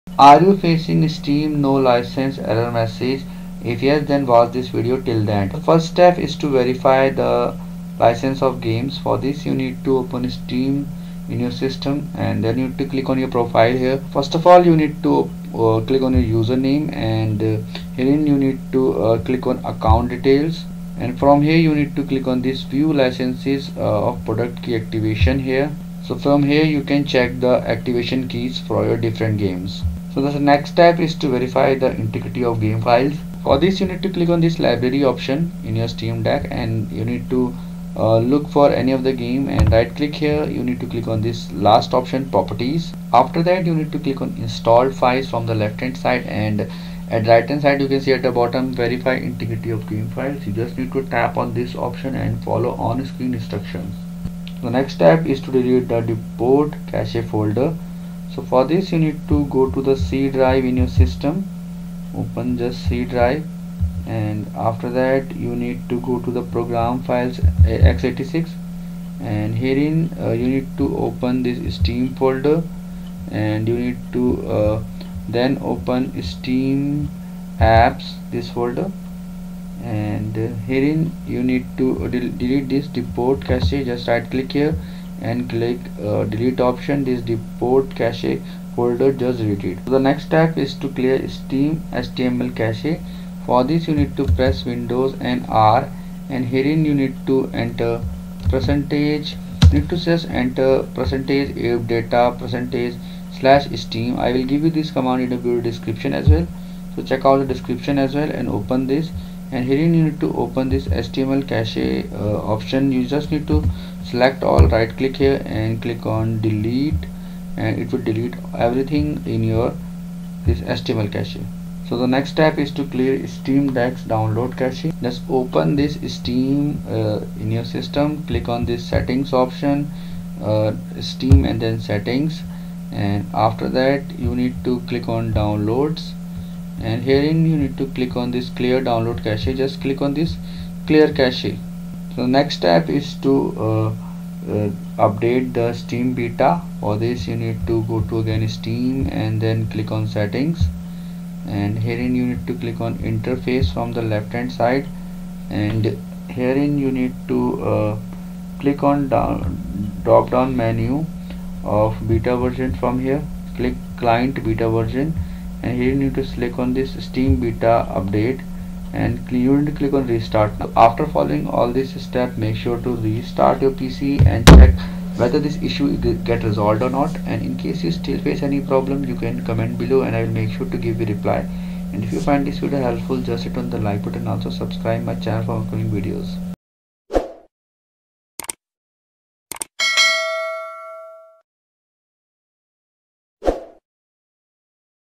[6 Ways To Fix] Steam No License Error While Launching Games
Are you getting an error occurred while launching this game no license crack in Steam Deck?

Step 1 - Verify The License of Games

• Open Steam Deck in your system
• Click on your username from the top right and select “Account details”
• Click on “View licenses and product key activations”

Step 2 – Verify Integrity of Game Files In Steam

• Go to Steam Deck
• Click on “Library”
• Right click over the game for which you want to verity integrity and select “Properties”
• Click on “Installed Files” and then choose “Verity Integrity of game files”

Step 3 - Delete Depotcache folder

Close Steam & go to "C:\Program Files (x86)\Steam/steamapps" (or wherever your steam is installed in) and delete everything in the "depotcache" folder.

Step 4 – Clear Steam HTML Cache

• Press Windows + R
• Enter %localappdata%\Steam and press OK
• Open htmlcache and delete everything

Step 5 - Clear Steam Deck’s Download Cache

• Click on “Steam” and select “Settings”
• Select “Downloads”
• Click on “Clear Cache” in front of Clear Download Cache

Step 6 - Steam Beta Update

• Select “Interface”
• Click on drop down in front of Client Beta Participation and select “Steam Beta Update”

An error occurred while launching this game no licenses fix, Failure no licenses Steam family sharing, Failure no licenses steam,